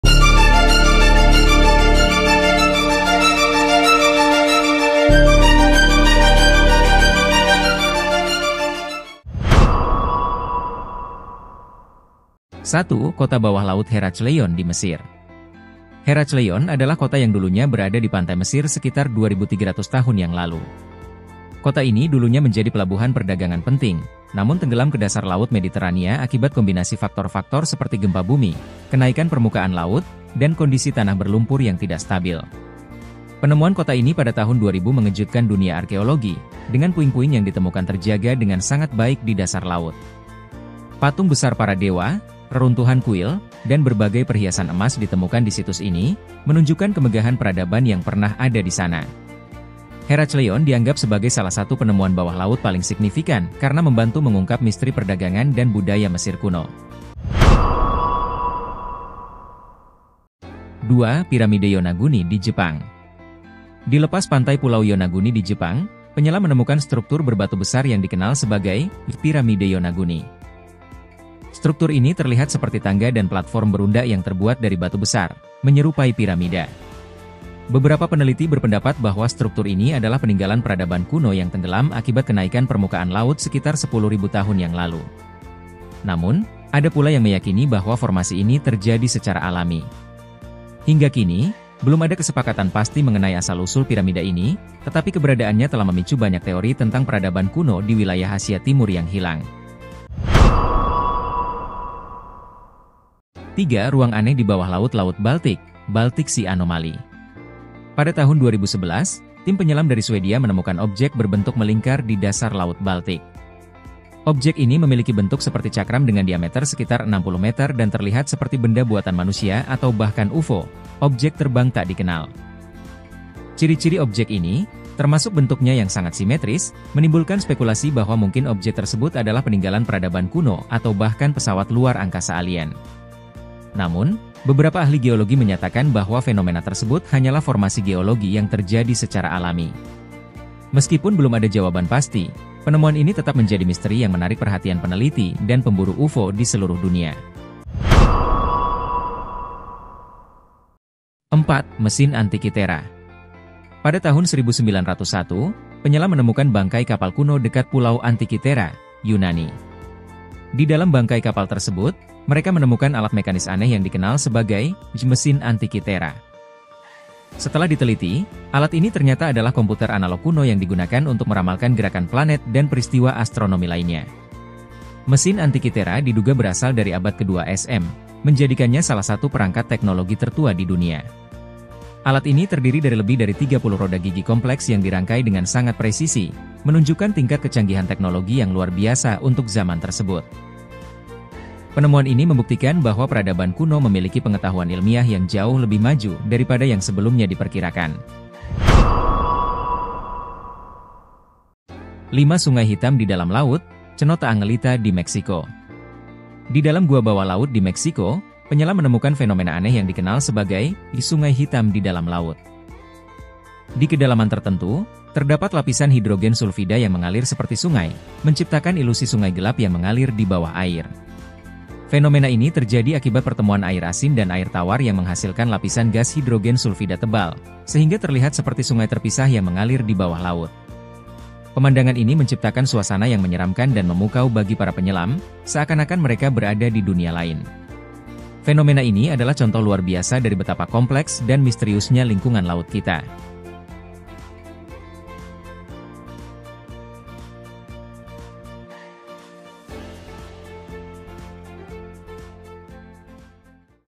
penemuan bawah laud yang menghebohkan dunia
Vidio ini menunjukkan lima penemuan bawah laud yang menghebohkan dunia hingga sulit di temu akal